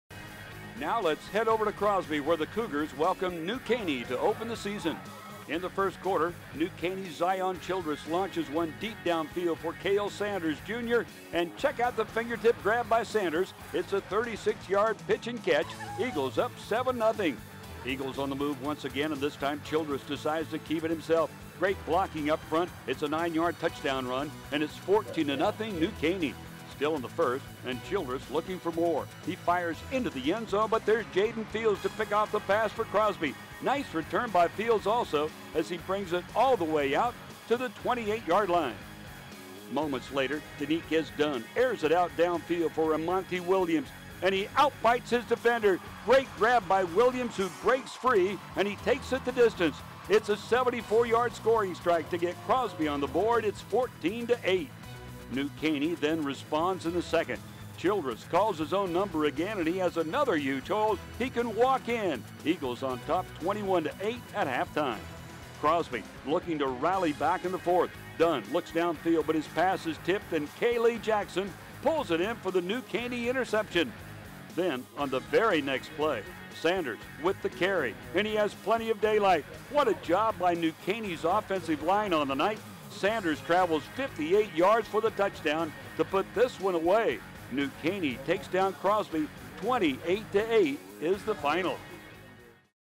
New Caney vs. Crosby - 2019 Week 1 Football Highlights
Let’s head over to Crosby, where the Cougars welcome New Caney to open the season…

In the first, New Caney’s Zion Childress launches deep downfield for Cale Sanders Jr, and check out the fingertip grab by Sanders. 36 yard pitch and catch, Eagles up 7-0.

Eagles on the move once again, and this time Childress decides to keep it himself. Great blocking up front and Childress trots in untouched, 9 yard score and it’s 14-0 New Caney.

Still in the first, Childress looking for more, he fires into the end zone, but there’s Jaden fields to pick off the pass for Crosby. Nice return by fields also, as he brings it out all the way to the 28 yard line.

Moments later, Deniquez Dunn airs it out downfield for E’Monte williams. Nice grab by Williams and he breaks free from the defender, and he takes it the distance. 74 yard scoring strike gets Crosby on the board, it’s 14-8.

New Caney responds in the second. Childress calls his own number again, and he has another huge hole. He can walk in, 17 yards untouched, Eagles on top 21-8 at halftime.

Crosby looking to rally back in the fourth, Dunn looking downfield but his pass is tipped, and Kay-lie Jackson pulls it in for the New Caney interception.

Then on the very next play, Sanders gets the carry, and he has plenty of daylight. What a job by New Caney offensive line all night, Sanders travels 58 yards for the touchdown to put this one away, New Caney takes down Crosby, 28-8 is the final.